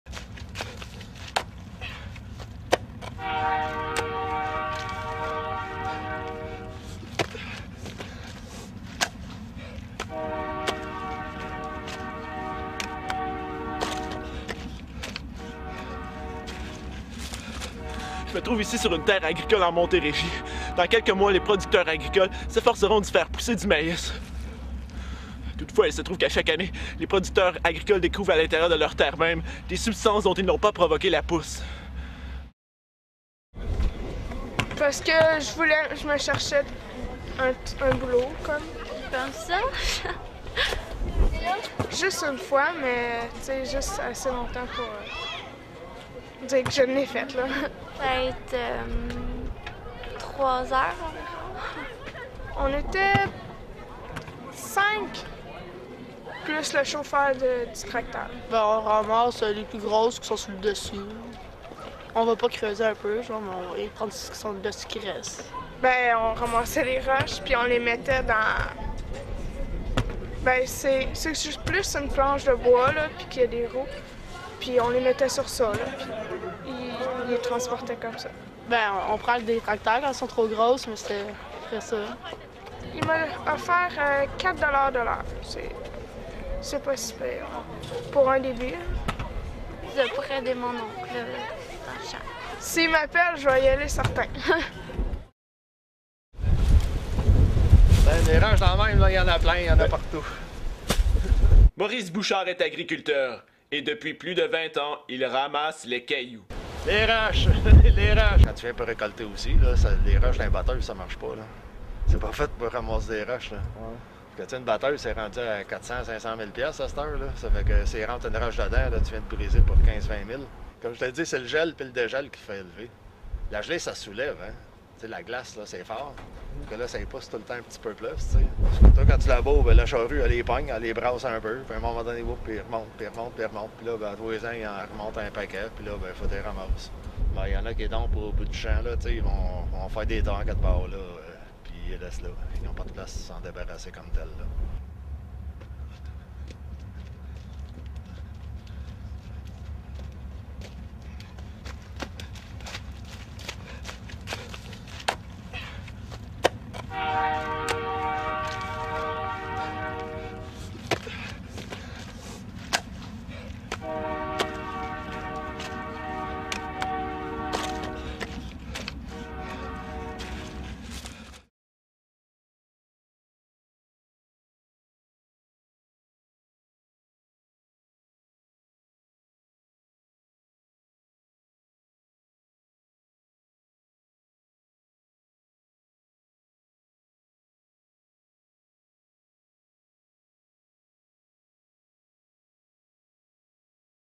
Doux Printemps
Reportage sur la pousse des roches en Montérégie.

2006